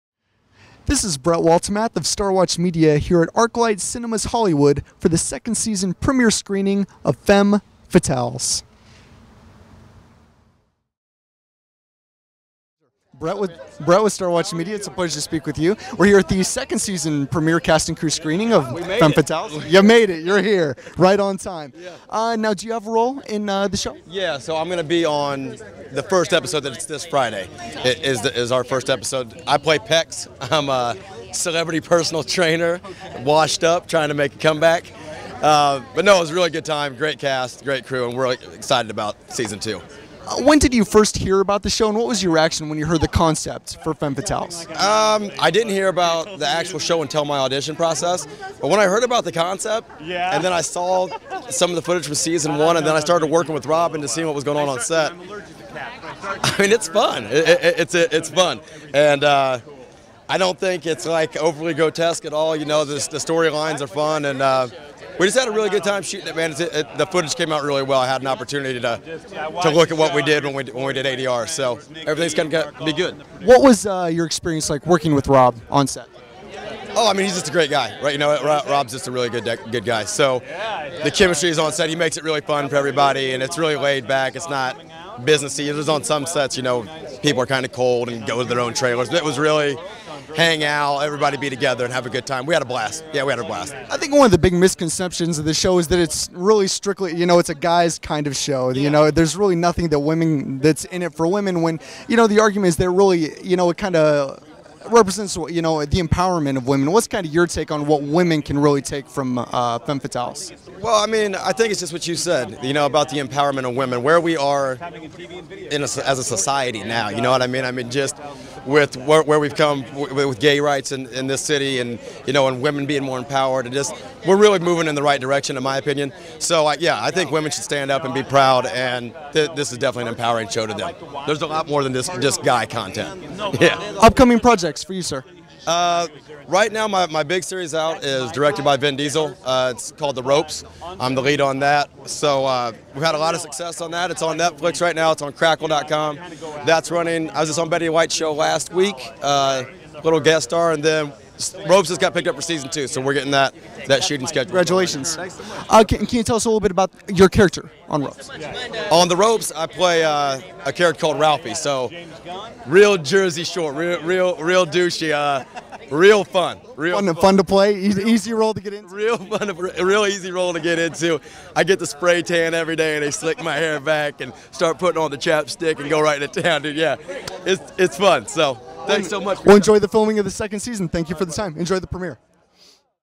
Joel Rush red carpet interview at "Femme Fatales" season 2 premiere screening
Actor Joel Rush attends the "Femme Fatales" second season cast and crew premiere screening at ArcLight Hollywood.

Mr. Rush discusses his character in an upcoming episode of "Femme Fatales," his reaction when first hearing about the series, his experience on set, misconceptions of "Femme Fatales" being a guy's type of show, other projects, and more.

Camera Operator: Jayson Lobo